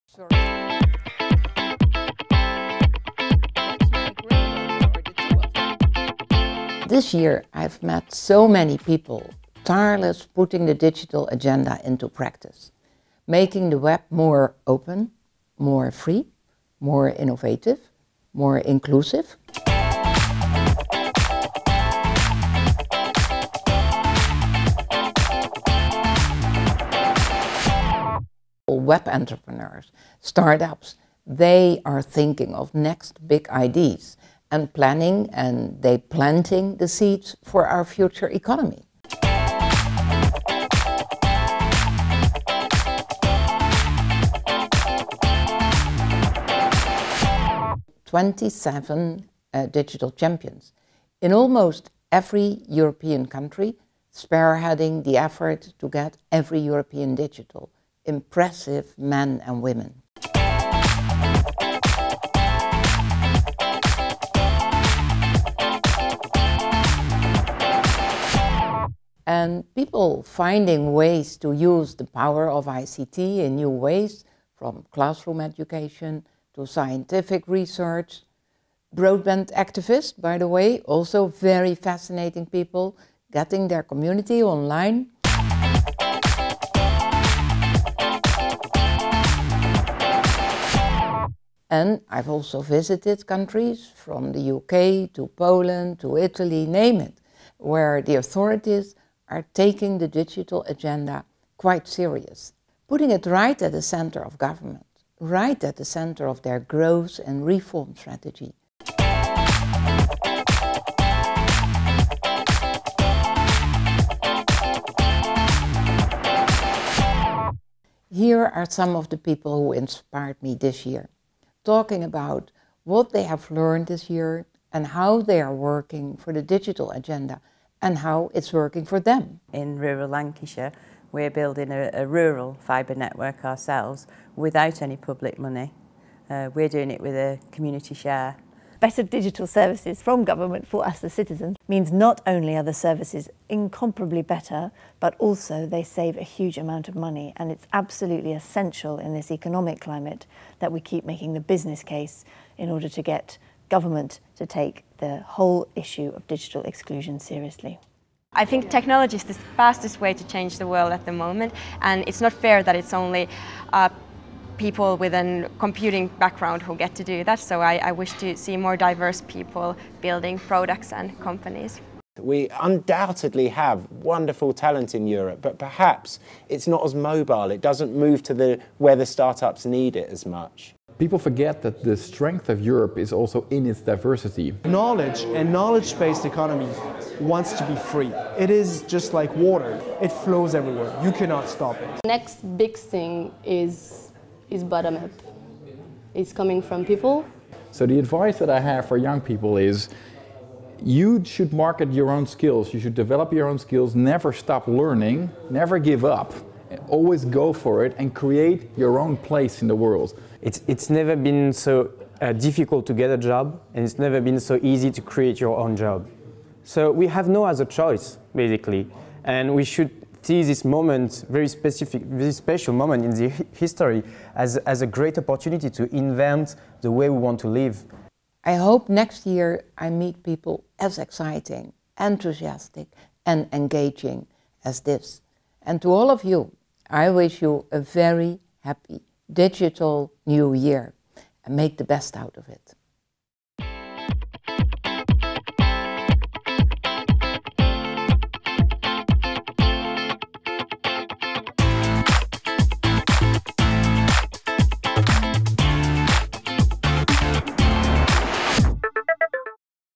It's all about people. Here's some of you who inspired me this year.
Neelie Kroes New Year Message 2013